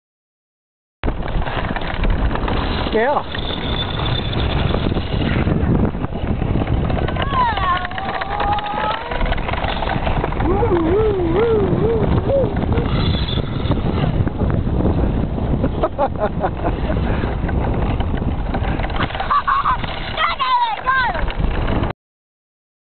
Kiddie iceboat
Bob outraces his dad. Videography done by Brooks Martin, Bozeman, Montana